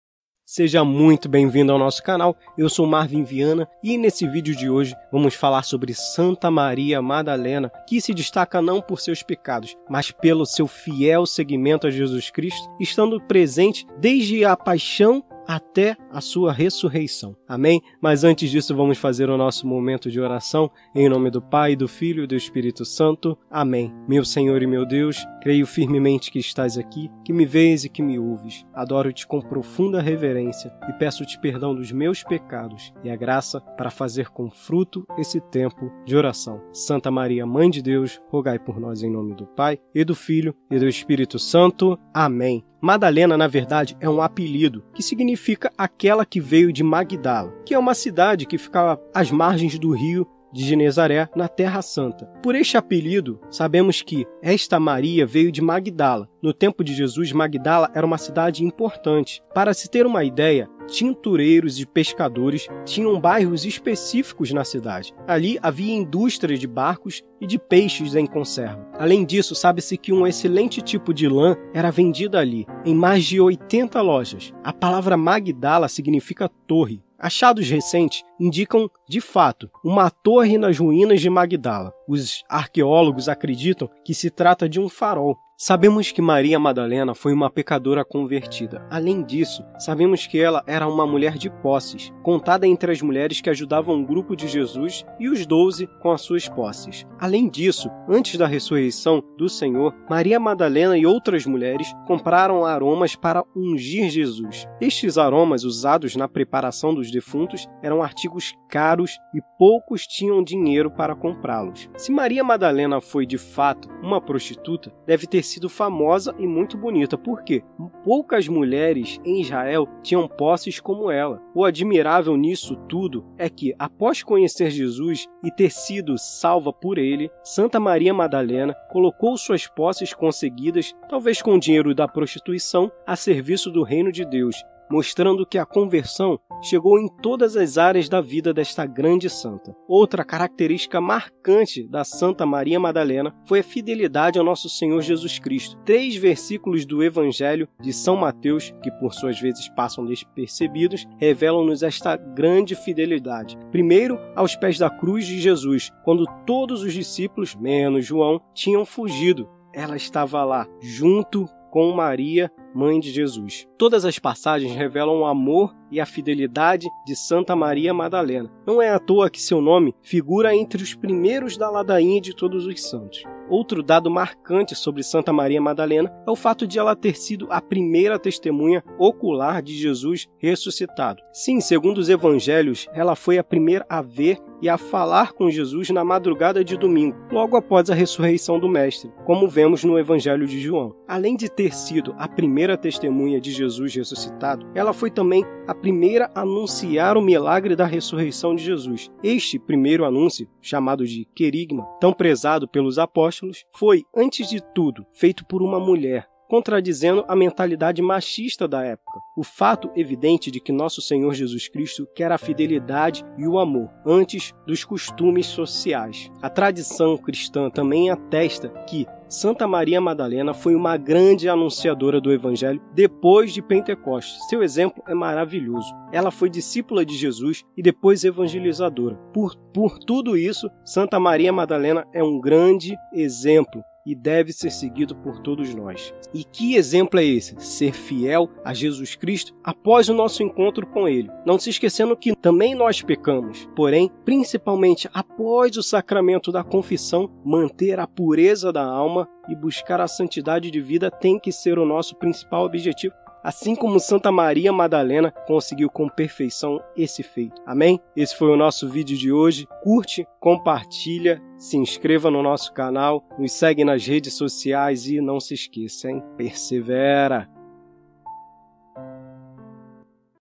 A fidelidade é realmente possivel nos nossos dias atuais? Descubra com Santa Maria Madalena
Santa Maria Madalena, nascida em Mágdala, na Galileia, é a mulher de quem, por obra de Jesus, saíram 7 demônios. Os 4 Evangelhos dão testemunho dessa fiel discípula de Jesus, que O acompanhou durante Seu ministério, se colando à disposição do Senhor para ouvi-l'O e servi-l'O, e seguiu o Mestre até a Cruz, como nos relata o Evangelho de São João: “Junto à cruz de Jesus estavam de pé sua mãe e a irmã de sua mãe, Maria de Cléofas, e Maria Madalena” (Jo 19,25). Ao ressuscitar, Jesus apareceu a Santa Maria Madalena e lhe deu o mandato de anunciar aos Apóstolos a maravilha que havia ocorrido. Santa Maria de Mágdala é um testemunho para nós de como o encontro com Jesus deve nos levar à conversão e ao seguimento do Senhor por toda a vida, na obediência ao que Deus nos pede.